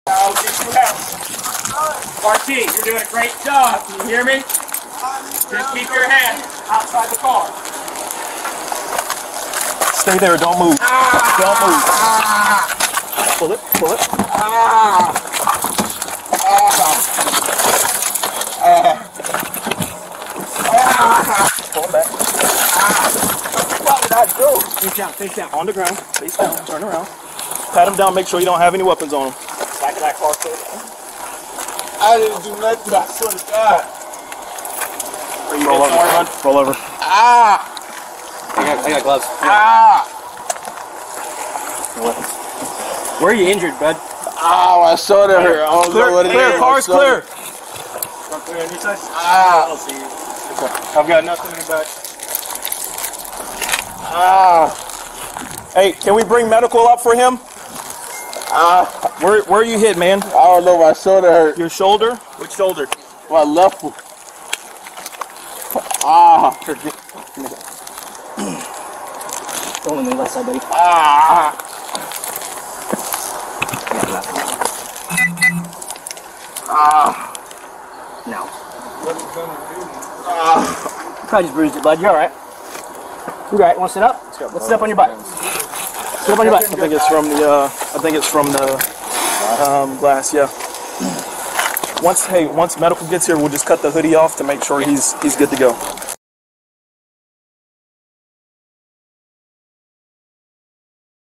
Body camera shows moments after deputies sprayed bullets into a patrol car (that had someone in it)
New body camera footage released from the Okaloosa County Sheriff's Office shows what happened after a deputy mistook an acorn falling for a gunshot. He (along with another deputy) shot into a patrol vehicle with a handcuffed, unarmed man. This is what happened after that shooting.